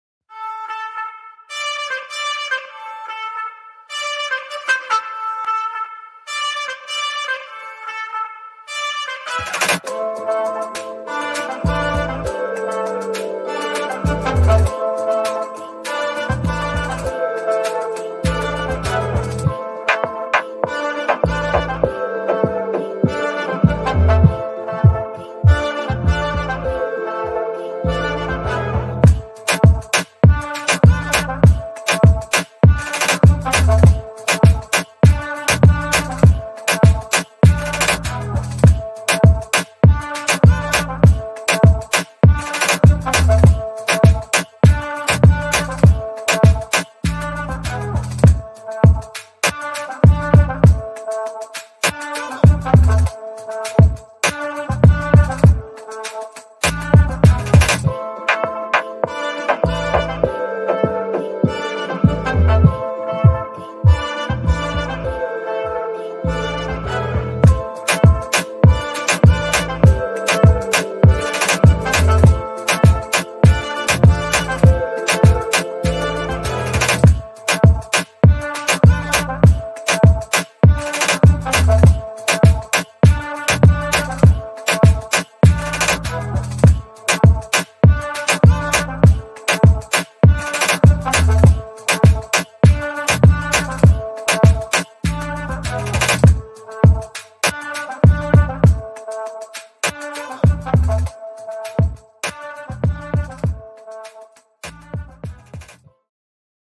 Create these adorable pig planters out of plastic bottles.
This adorable plastic bottle pig planter is a great addition to your garden, porch, or as a table centerpiece!